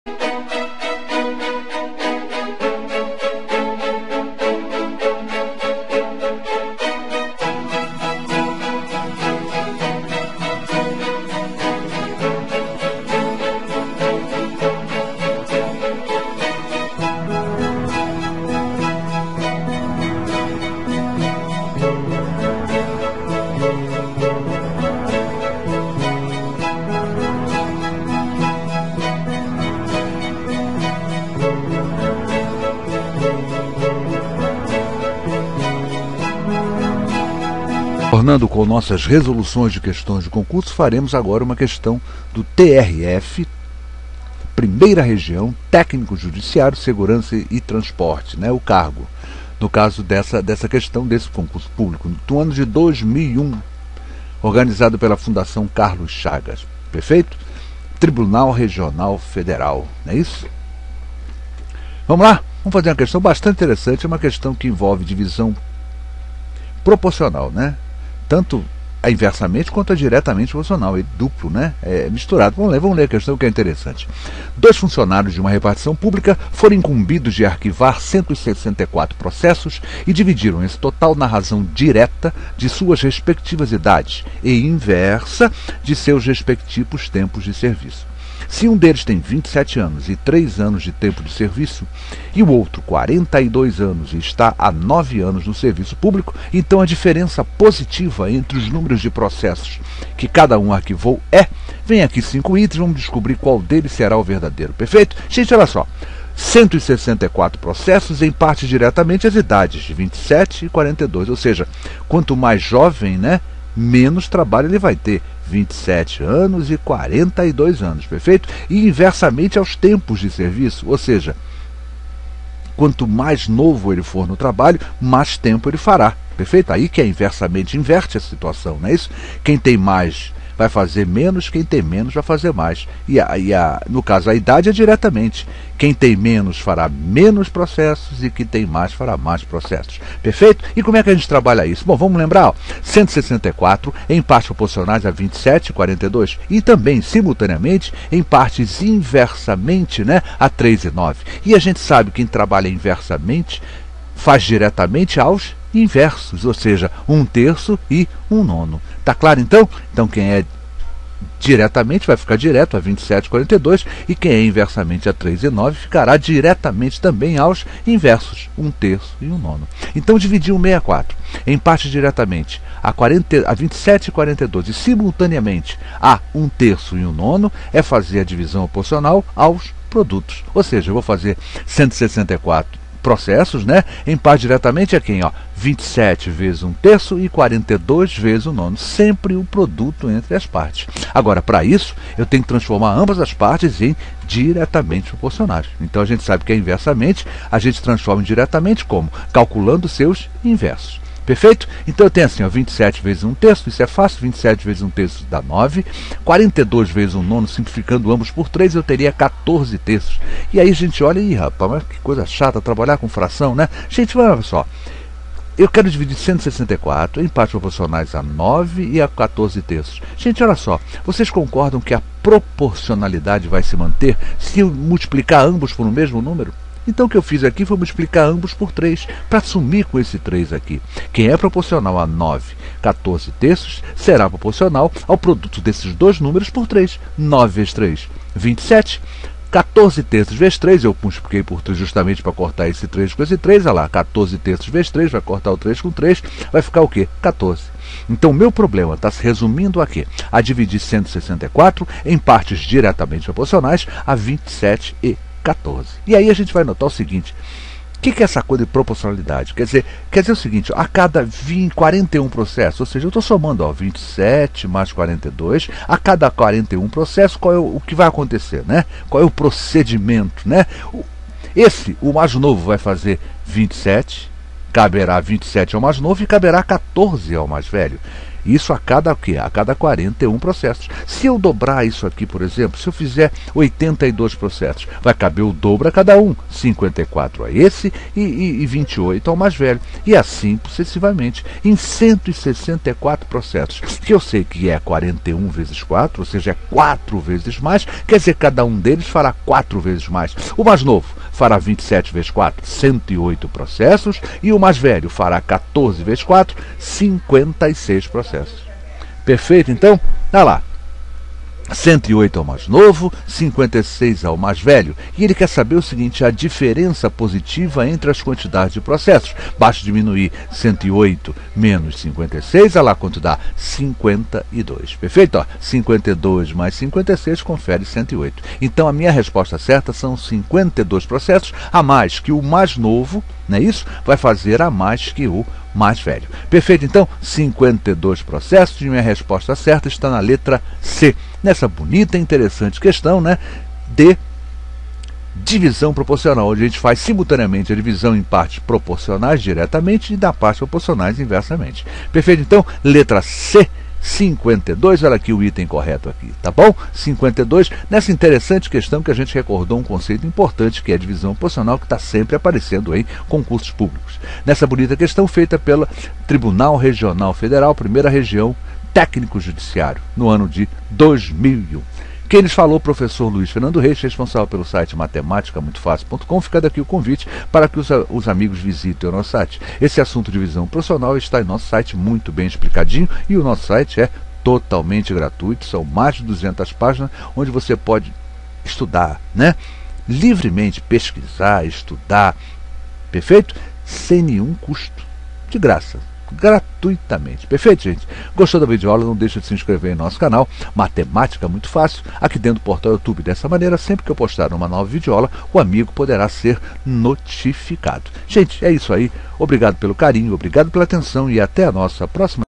TRF Divisão Proporcional Questão 04
TRF Divisão Proporcional Questão 04 - Videoaula Explicada pelo professor Luiz Fernando Reis do Vídeo educacional e livre para qualquer idade